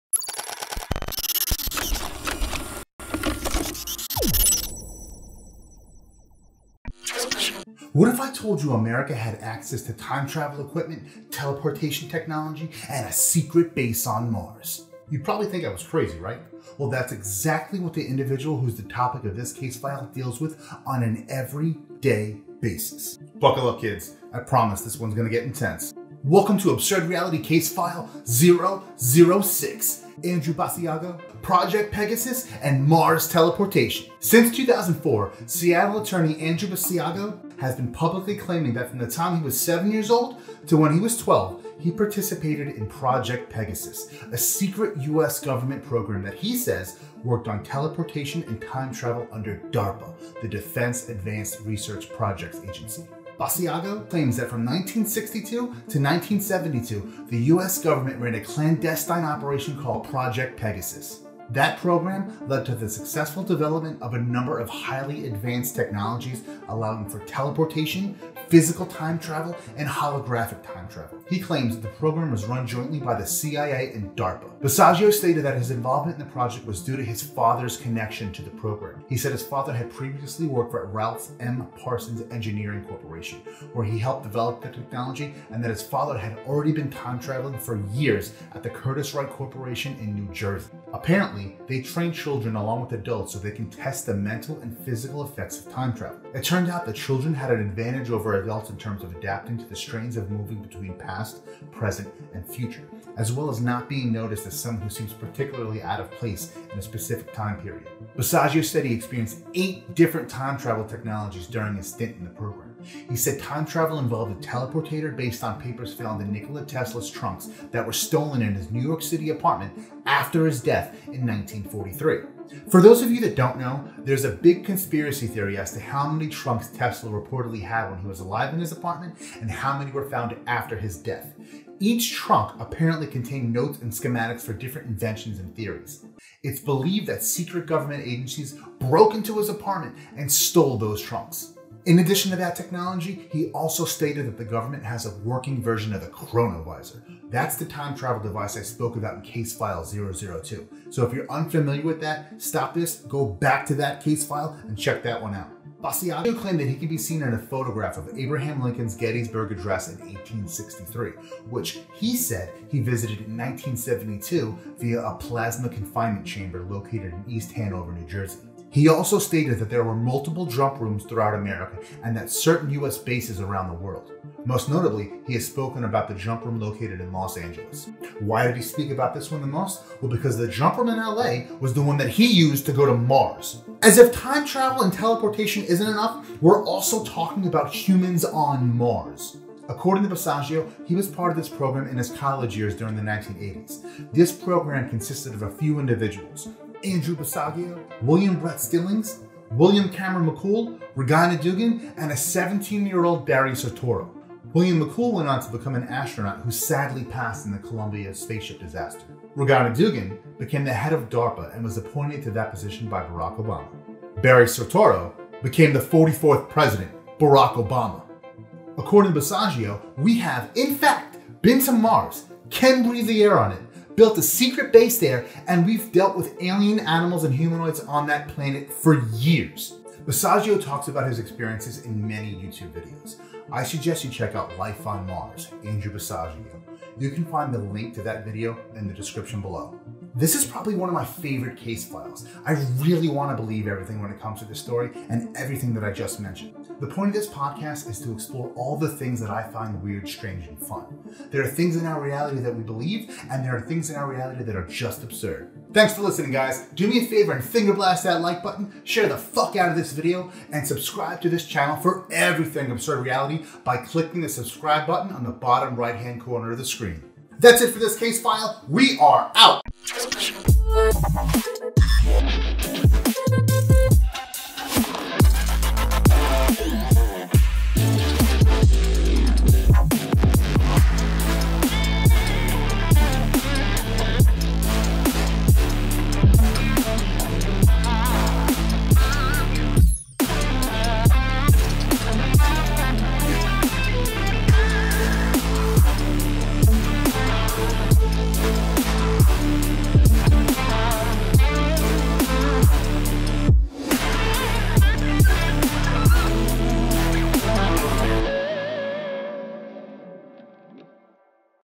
ABSURD REALITY: CASE FILE 6: Andrew Basiago, Project Pegasus, and Mars Teleportation
In this Case File host Joe Fiorello discusses the claims of Andrew Basiago and his history with Time Travel and Mars visitation.